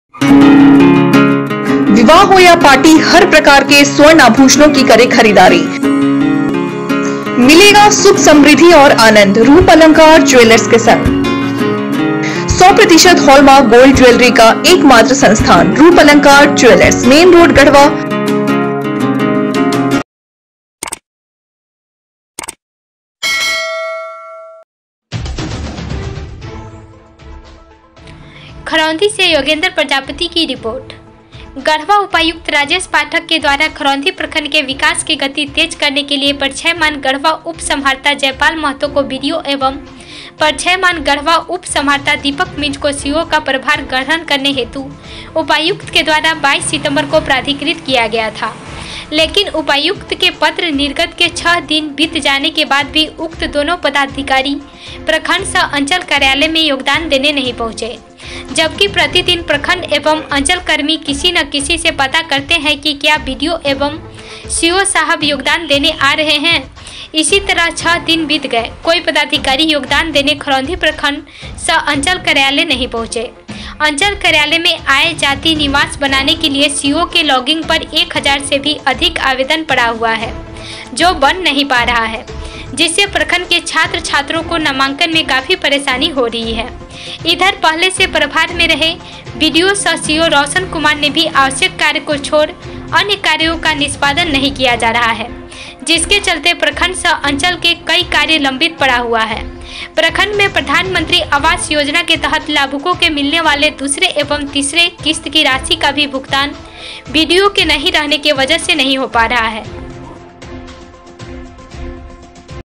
प्रखंड सह अंचल कार्यालय में योगदान देने नही पहुँचे पदाधिकारी, Kharaundhi news garhwa
kharaundhi garhwa news , kharuandhi news , kharaundhi breaking news , talash news kharaundhi , talash tv news kharaundhi.